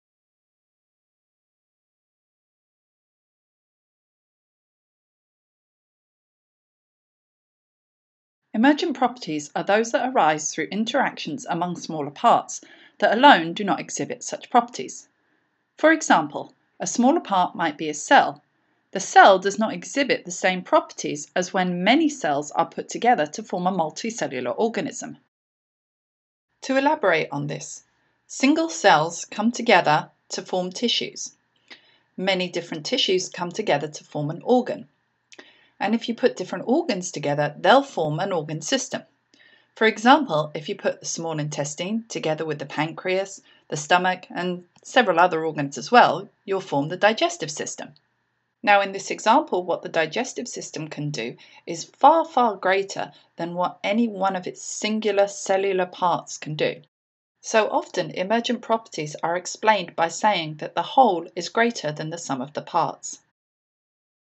1.1 Emergent Properties
What is an emergent property in Biology?  Why do we say that multicellular organisms have them?